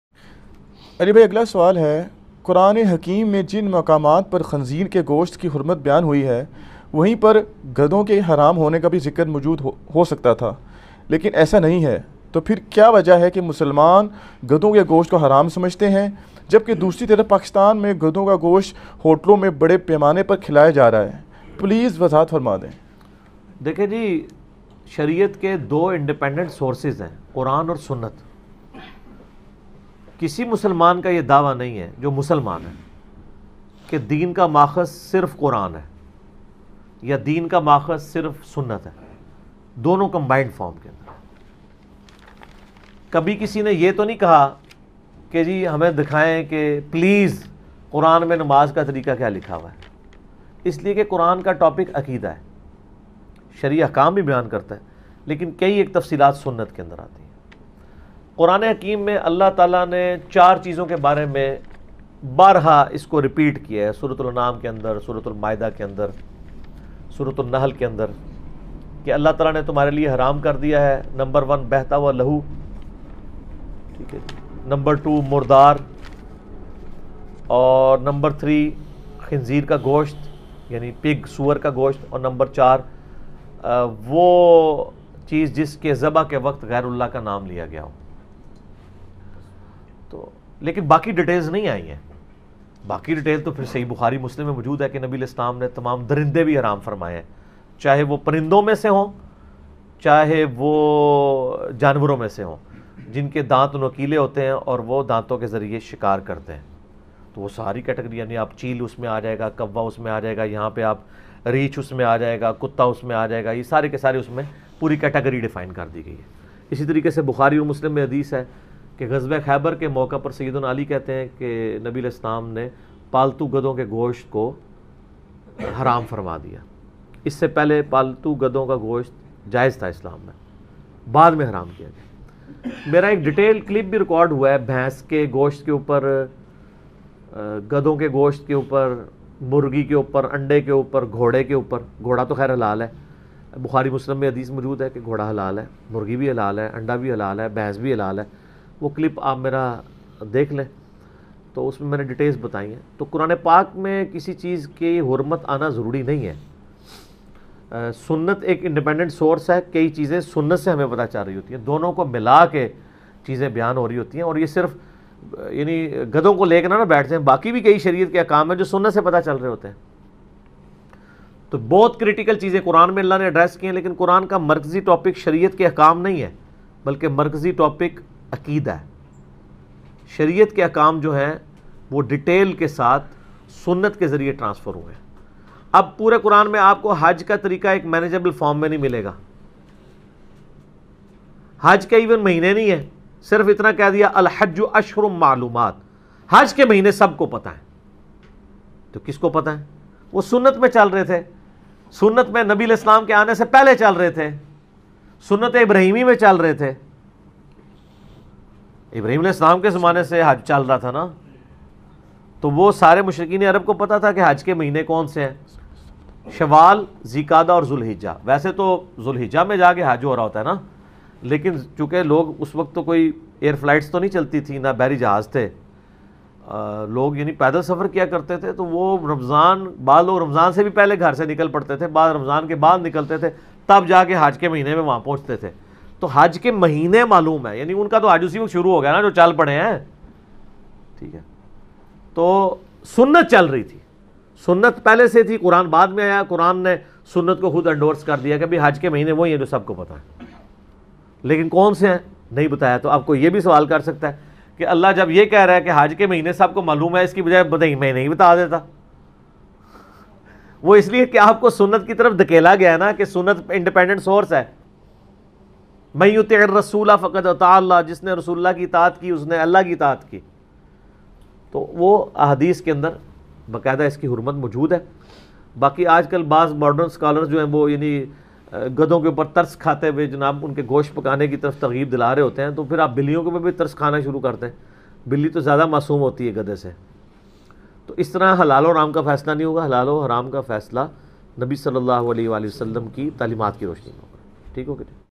Quran o Sunnat ky bary mai Wazahat | Deen Hasil karne k Sources | Ghady ka Gosht Halal ya Haram
Majlis 59

*Let's LEARN & Understand QUR'AN First!*